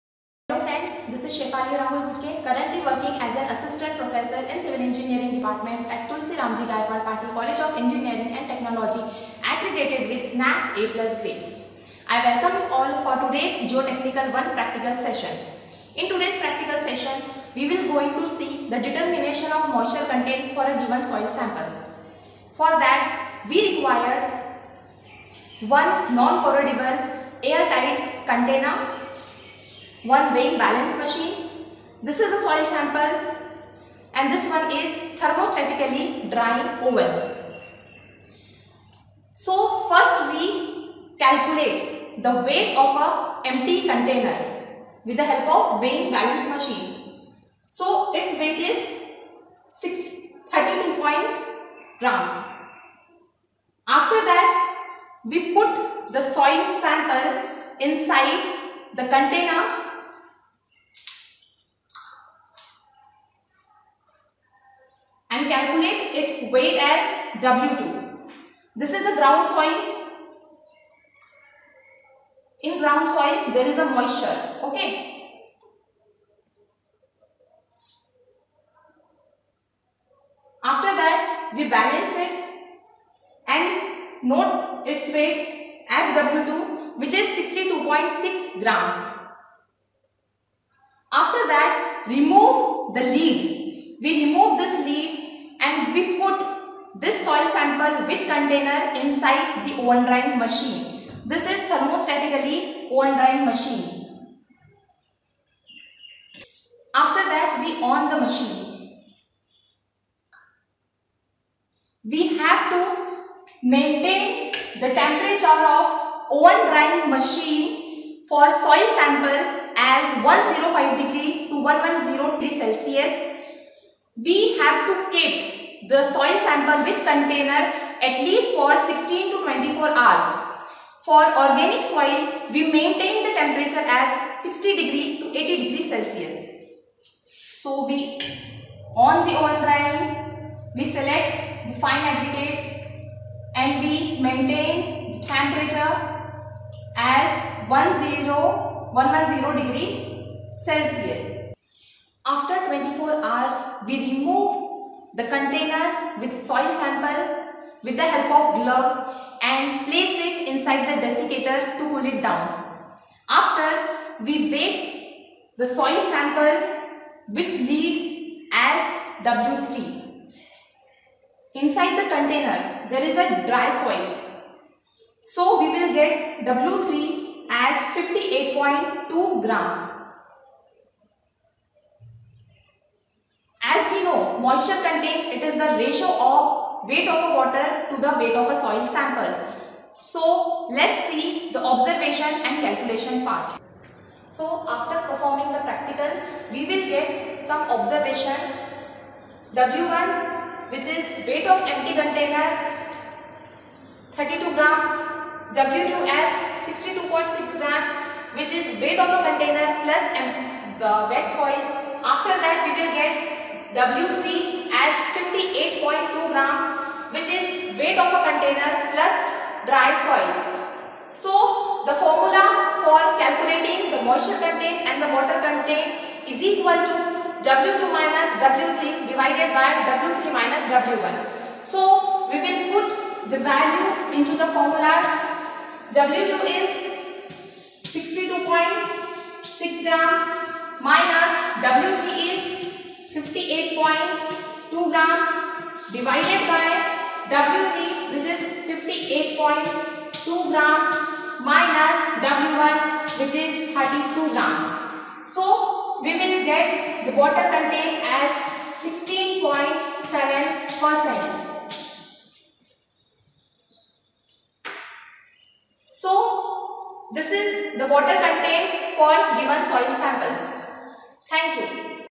Geotechnical Engineering-I: Determination of Moisture content of soil sample by Ms. Shefali Ghutke
Geotechnical Engineering-I: Determination of Moisture content for a given soil sample by Ms. Shefali Ghutke (Asst. Professor, Civil Engineering Department, Tulsiramji Gaikwad-Patil College of Engineering and Technology)